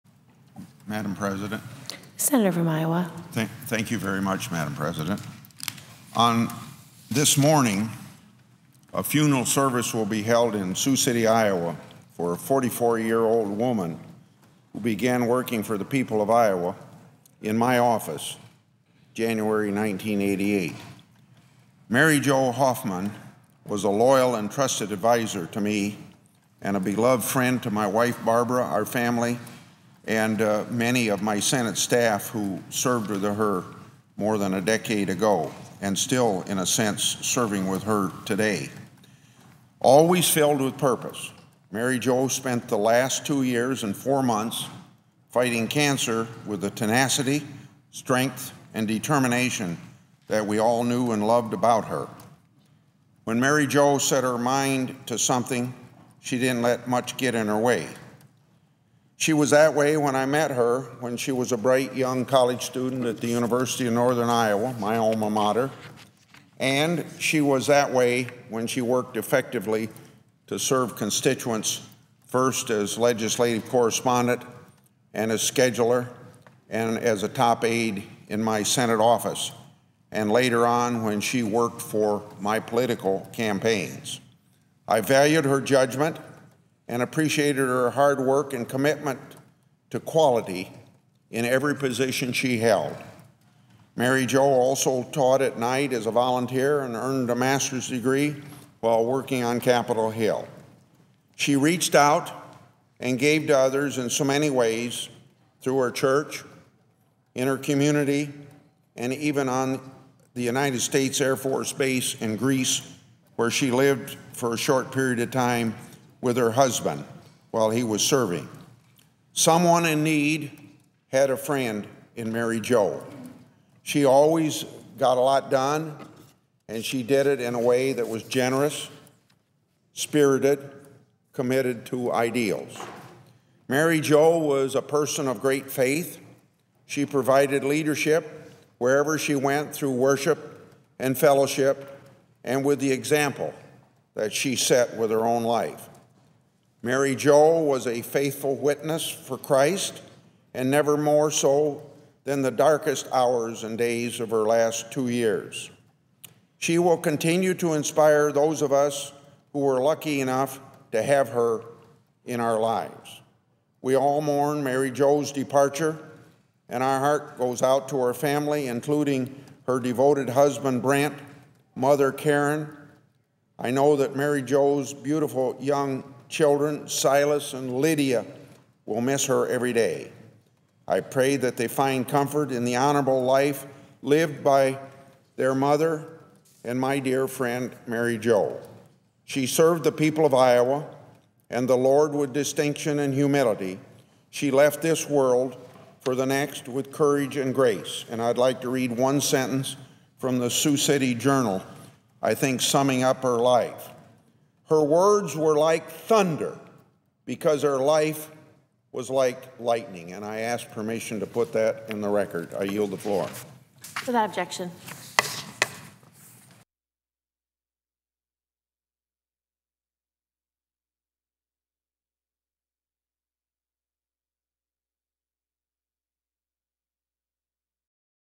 Senator Chuck Grassley - Tribute to Mary Jo Hoffman
A moving and memorable tribute to Mary Jo Hoffman from the U.S. Senate by Senator Chuck Grassley (R-IA).  Senator Grassley was like a second father to Mary Jo, and she served on his staff for many years.  Mary Jo Hoffman left a remarkable legacy of faith and kindness, and many friends and family to cherish her memory.  She was the wife of Brent Hoffman and mother of two children, Silas and Lydia.  She passed-on in July 2009 at the age of 44.